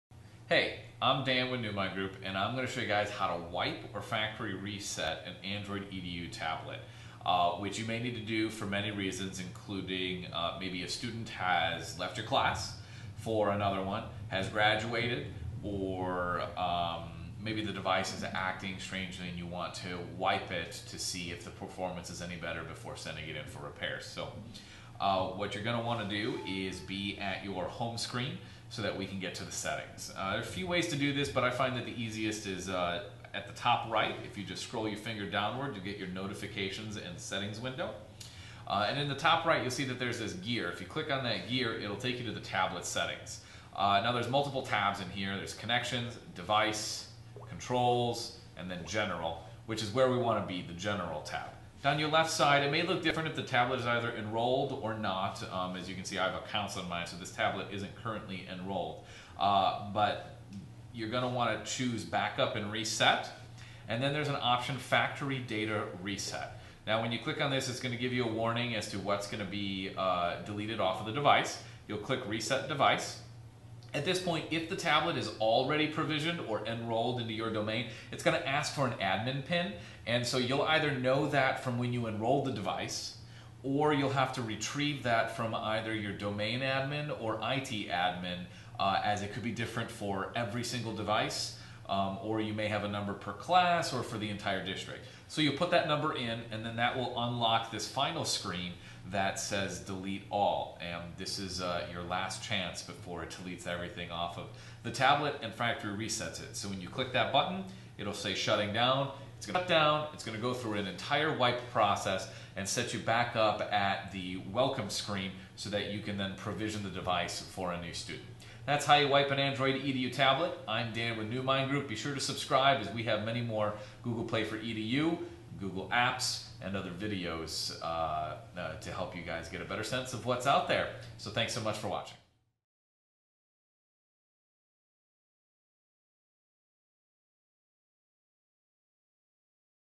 Factory Reset a Google Play for Education Tablet
Dan from Newmind Group walks through how to factory reset (wipe) a Google Play for Education tablet.